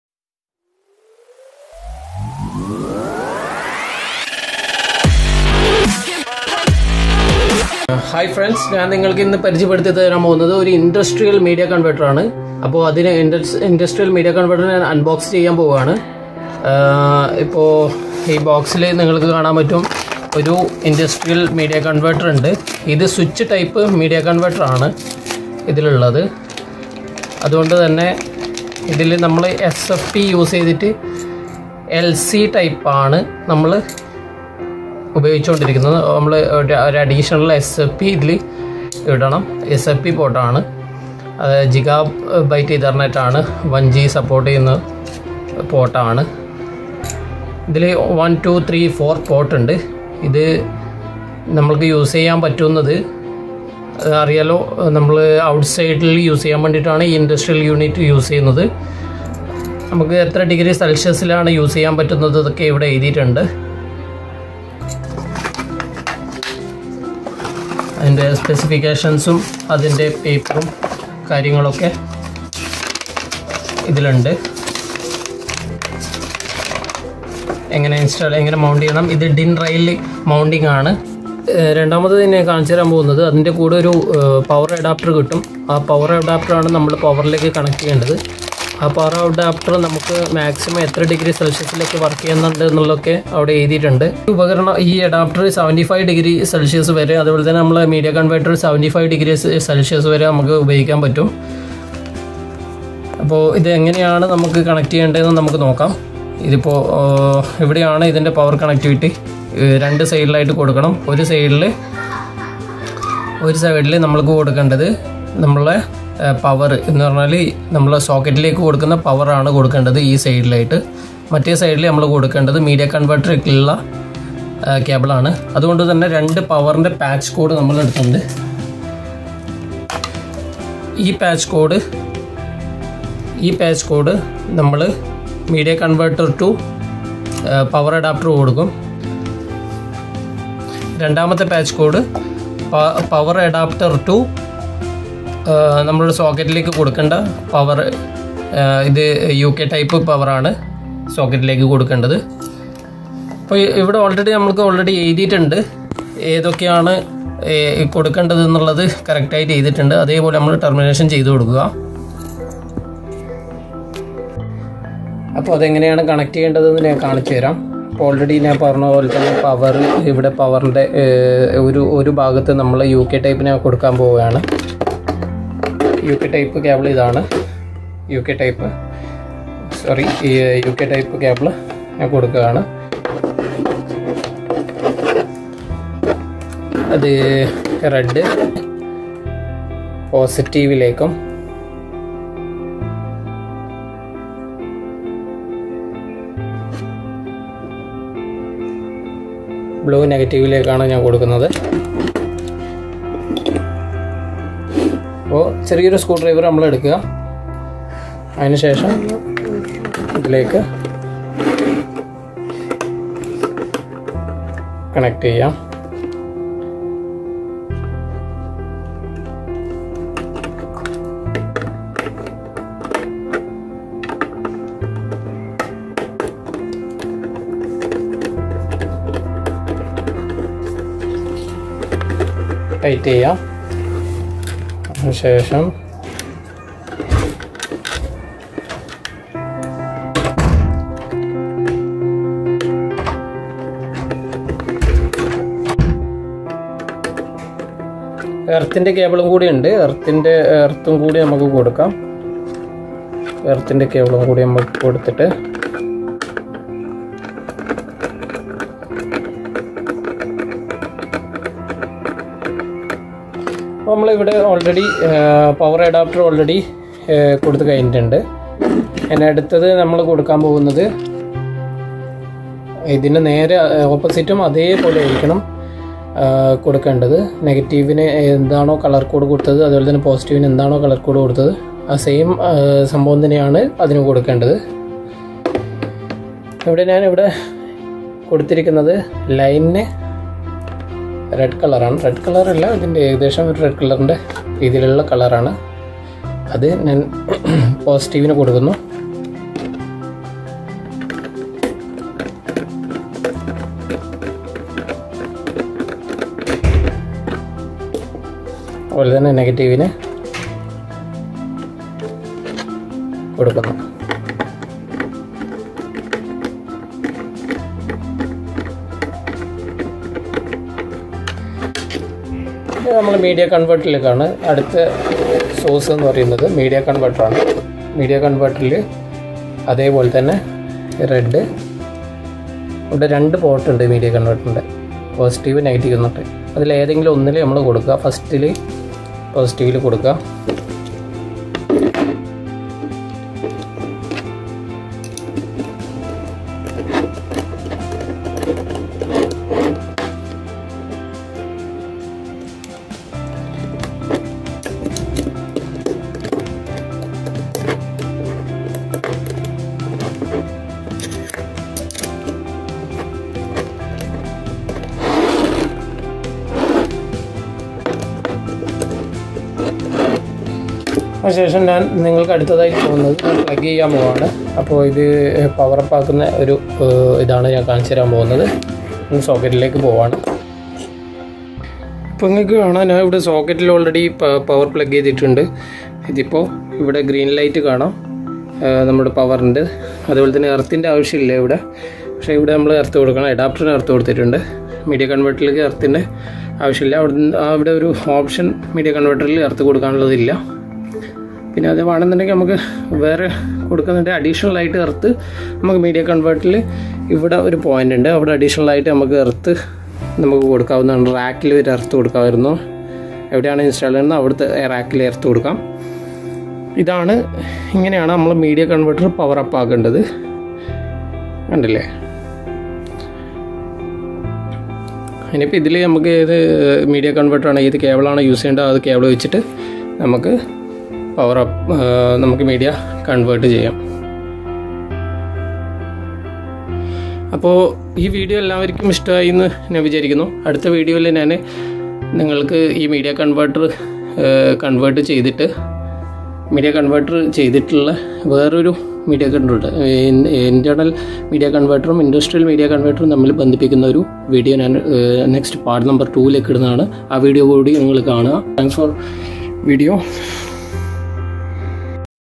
Unboxing industrial media convertor /malayalam
Industrial Media Converters are used to easily connect fiber cabling to different types of devices, cables or networks in locations that are exposed to harsh industrial operating conditions and high or low temperatures